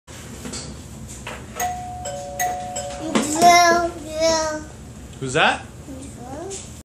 It's Joe!
Odin always wants it to be Joe when the doorbell rings.